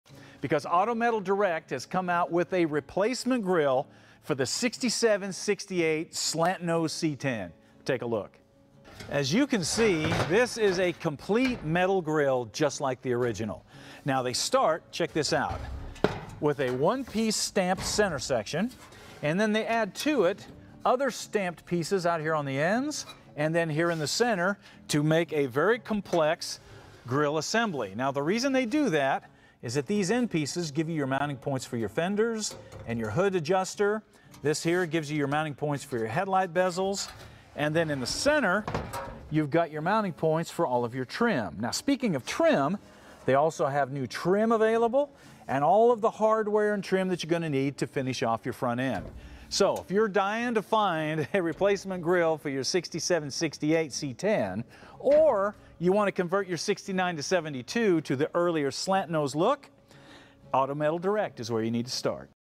Transform your 67 68 C 10 Auto Metal Direct's New Replacement Grill
@OfficialStaceyDavid gets it! This 67-68 ChevyC10 is get a facelift with yours truly